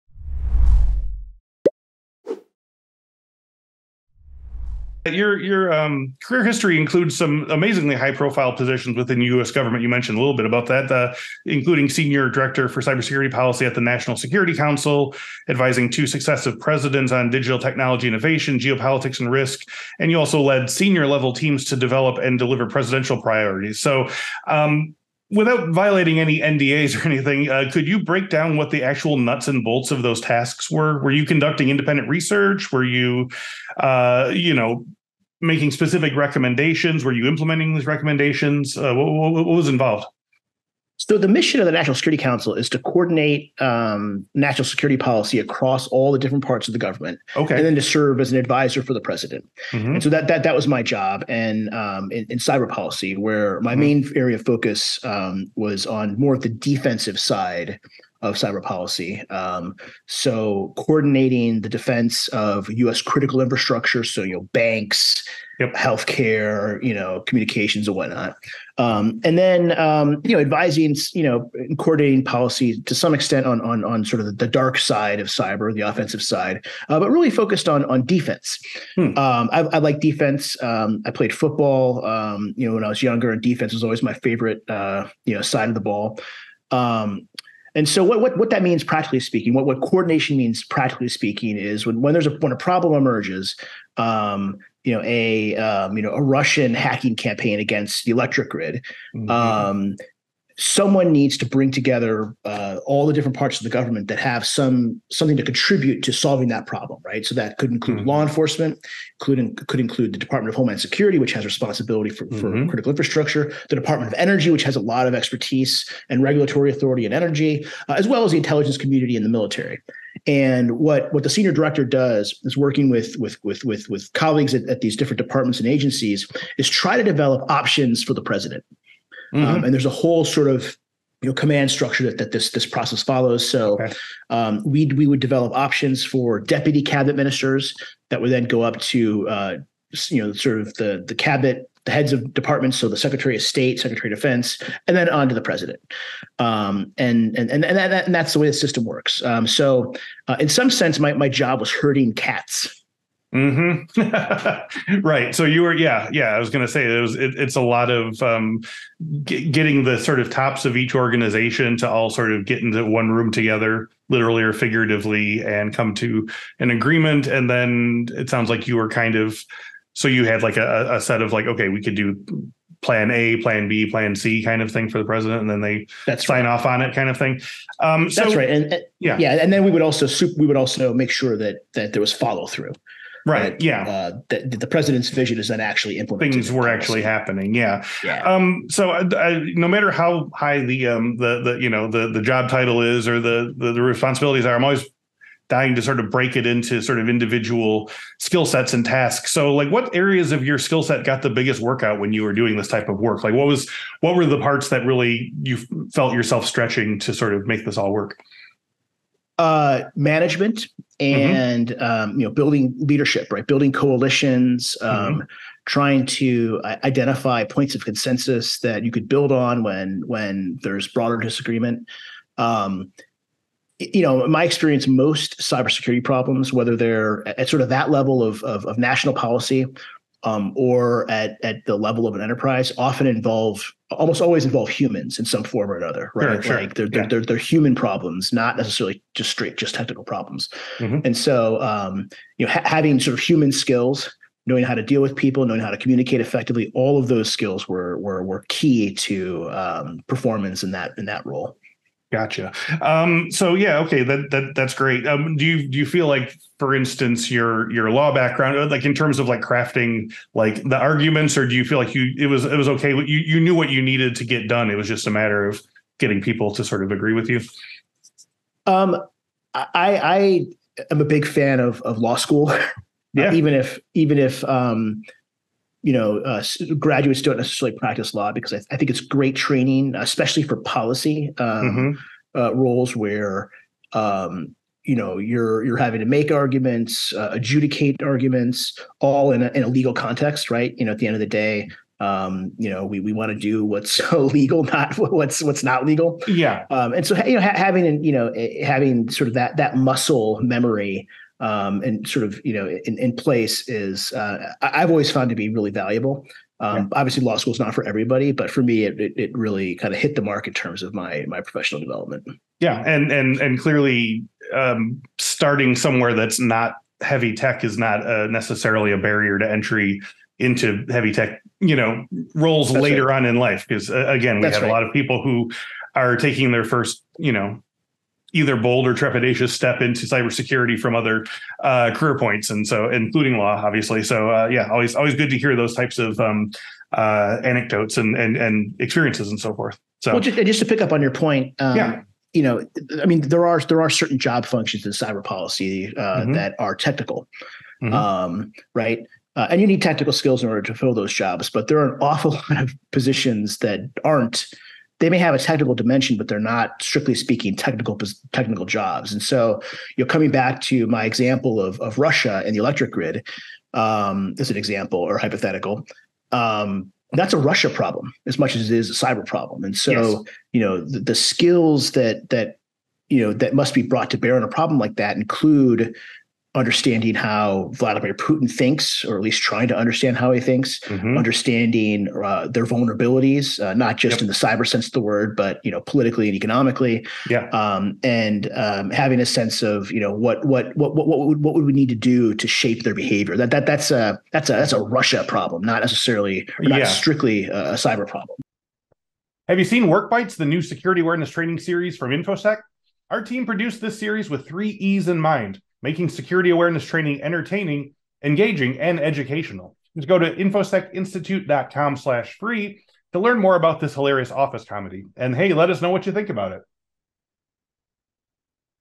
Cybersecurity advisement under two U.S. Presidents | Cyber Work Podcast
Guest AJ Grotto is the William J. Perry International Security Fellow and founding director of the Program on Geopolitics, Technology and Governance at the Stanford Policy Center and Stanford University. He breaks down the work he did at his most high-profile job, acting as cybersecurity advisor under two successive presidents, and explains how the process was often a bit like “herding cats.”